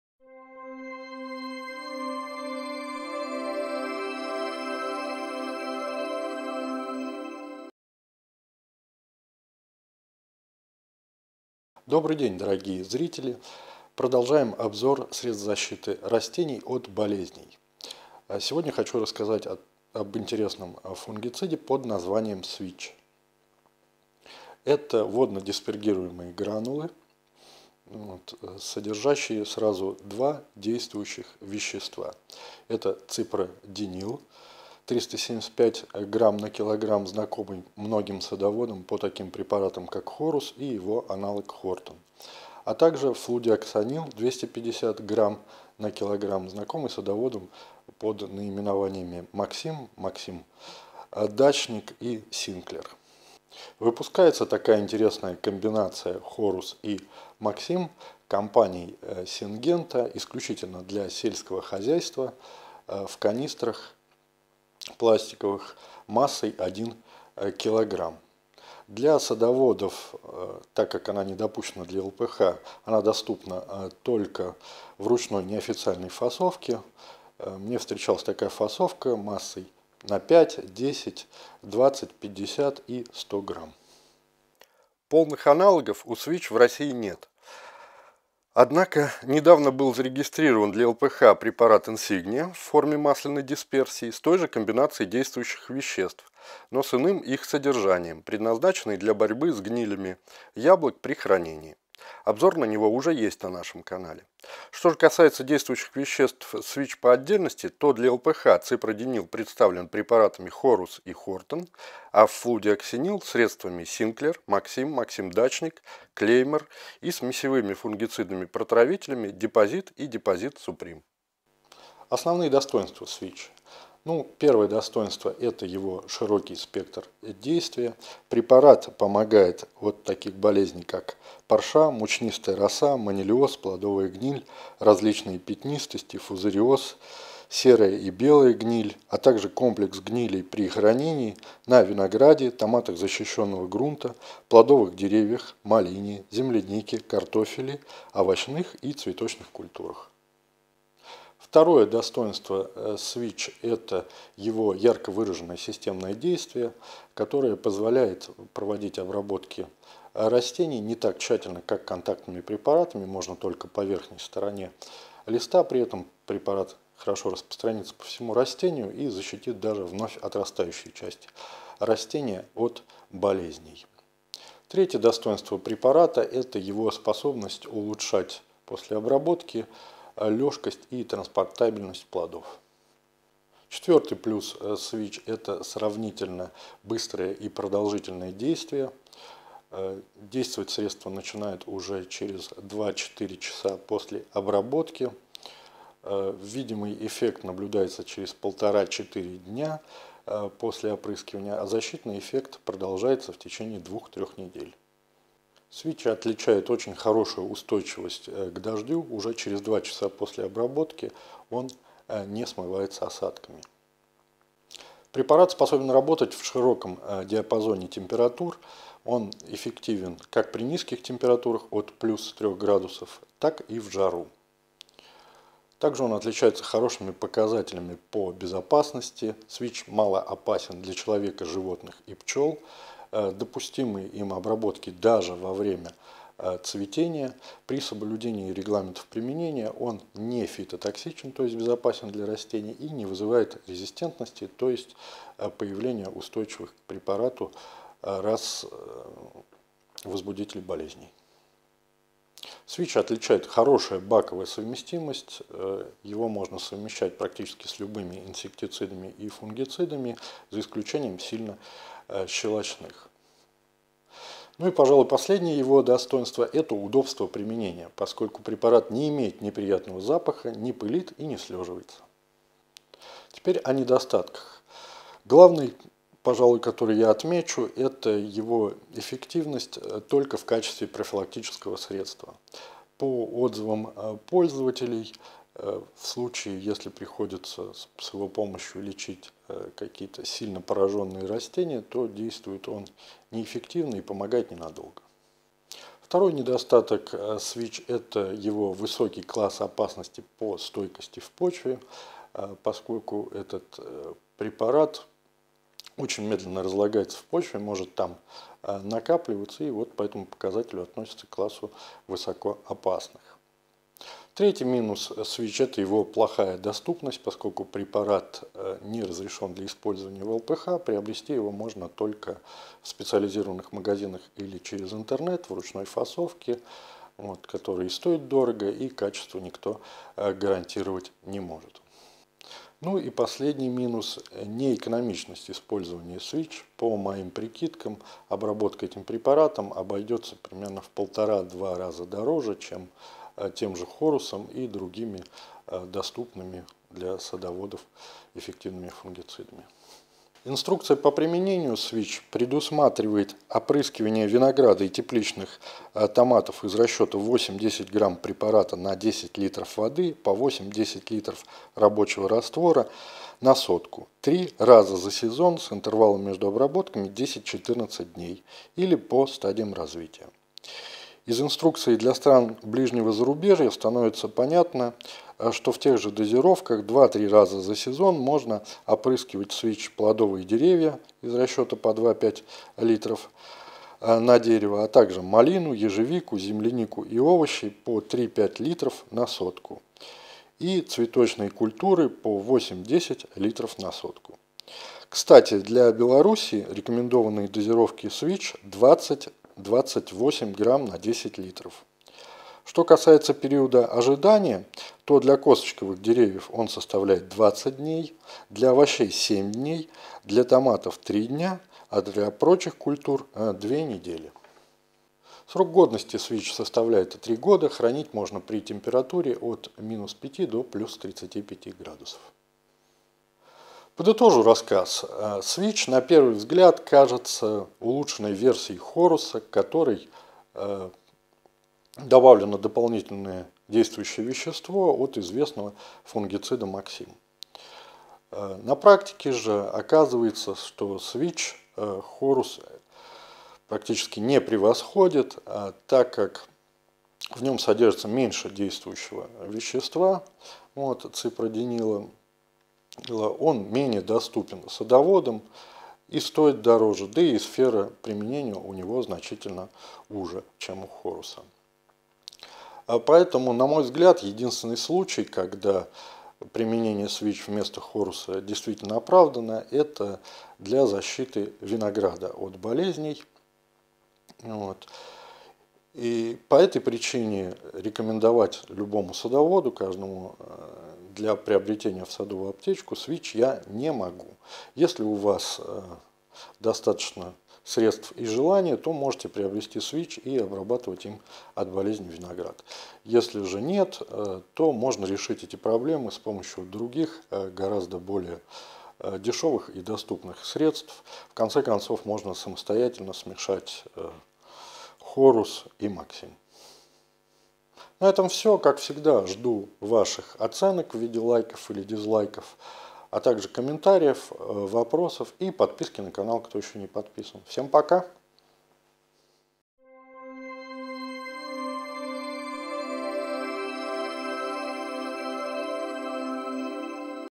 Фунгицид Свитч - надёжное средство от гнилей винограда, плодовых, ягодных и овощей. Обзор препарата
Свитч - смесевой фунгицид против разнообразных гнилей винограда, плодовых деревьев, ягодных кустарников и овощных культур. Обзор средства: формы выпуска, аналоги, сфера применения, достоинства и недостатки, регламент использования, условия хранения и срок годности.